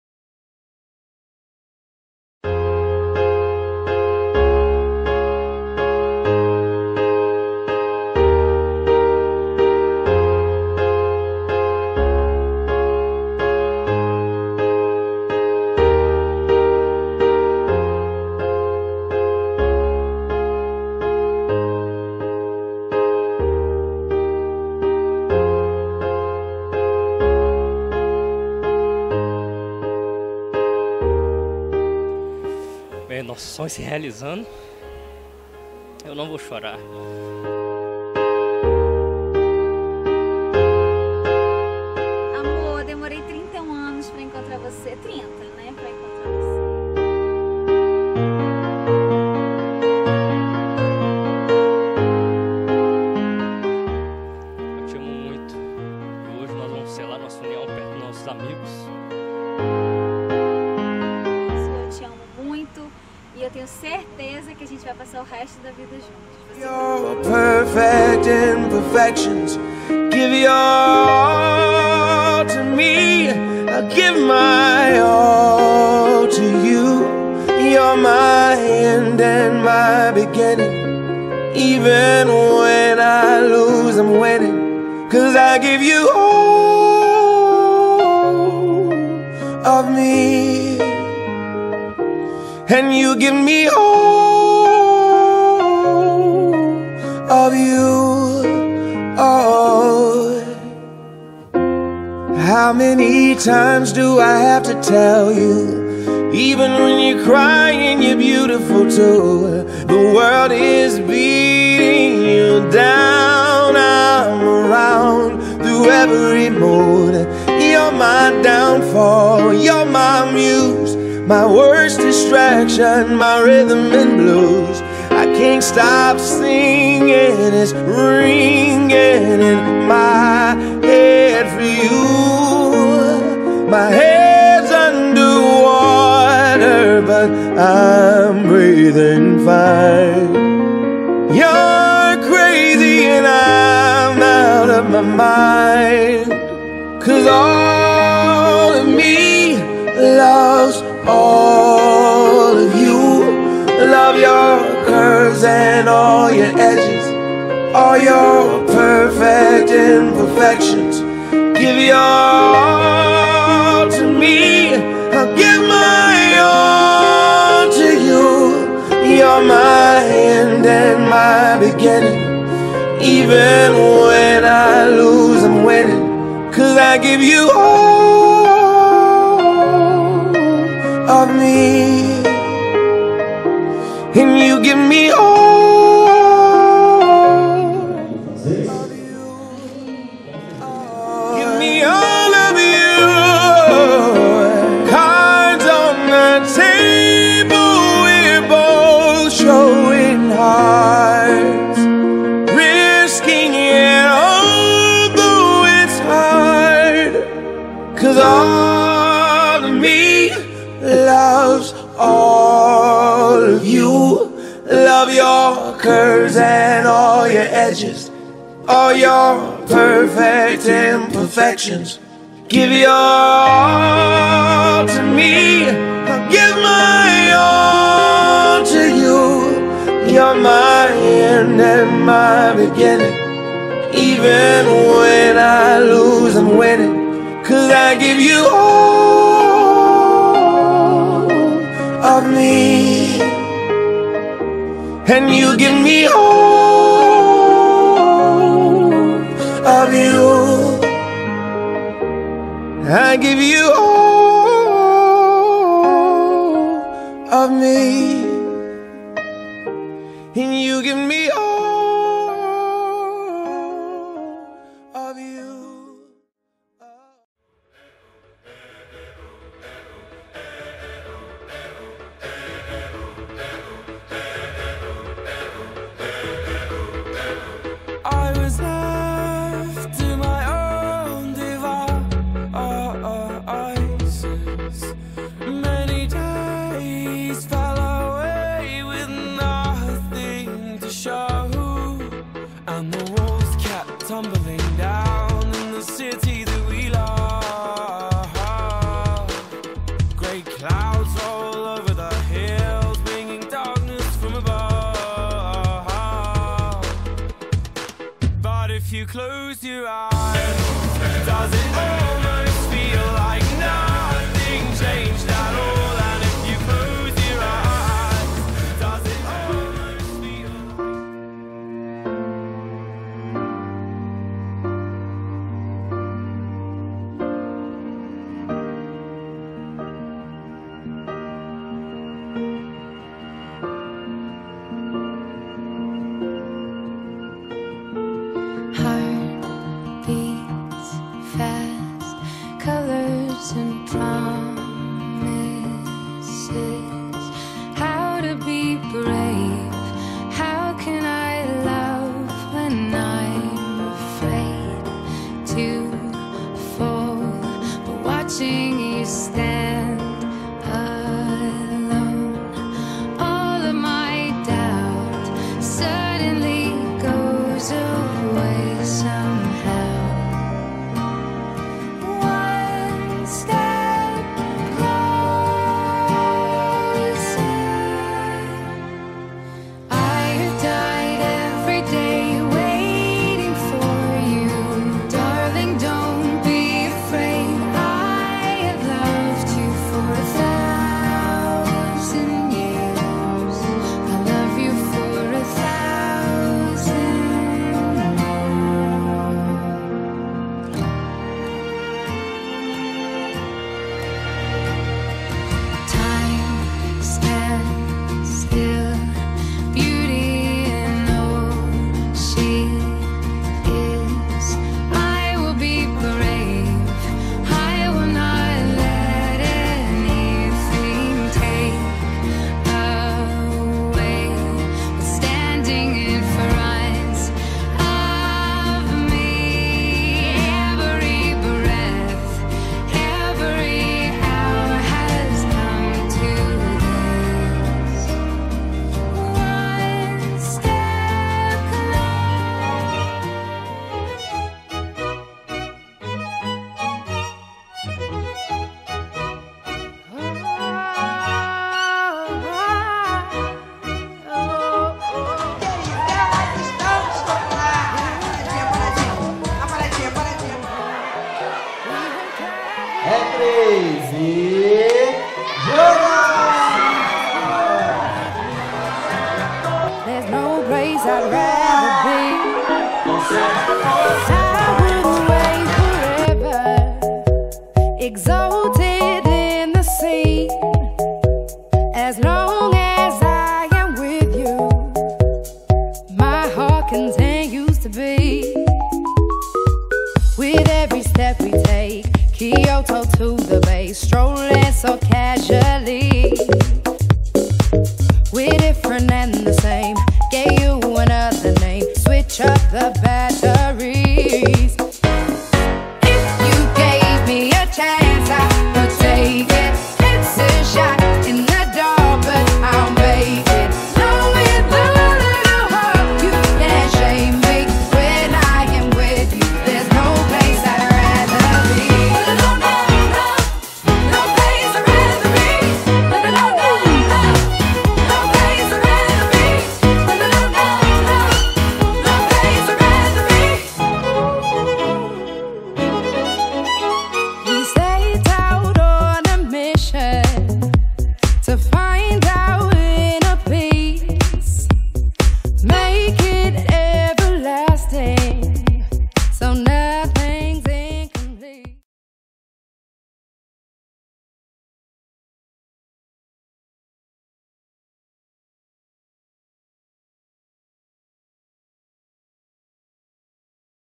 Manuela e Dolglas
Casamento Igreja Católica Santo Antonio, Praia da Costa, V.Velha-ES
Cerimonial Favoreto, Santa Paula, V.Vilha -ES.
Fotografia e Filmagem Bruno e Rodrigo Fotografia e Filmagem